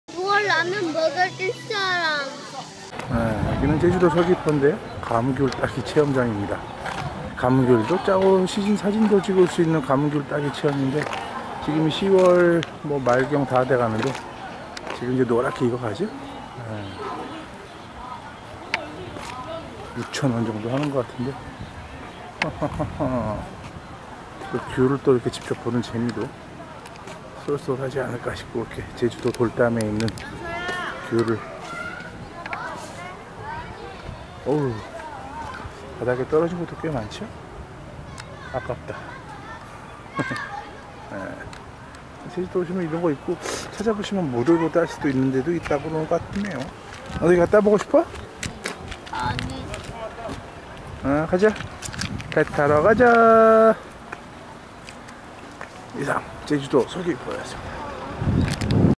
제주도 감귤따기 체험장, 귤을 직접 나무에서 딸수있는 곳과 가격 정보
제주도에 갔더니 귤을 나무에서 직접 체취하는 체험장이 있더군요.
그리 비싼 가격은 아닌데, 비닐봉지에 마음껏 담아서 가져올수 있다고 하는데, 저도 한번 해보고 싶지만 여행 일정상 그냥 통과를 했는데, 기회가 된다면 아이와 함께 따보고 싶다는 생각이 들기도 하더군요~
간혹 무료 체험을 할수 있는곳도 있다고 하는데, 제주도 여행을 가신다면 여행코스중에 하나 넣어보시는것도 좋지 않을까 싶습니다~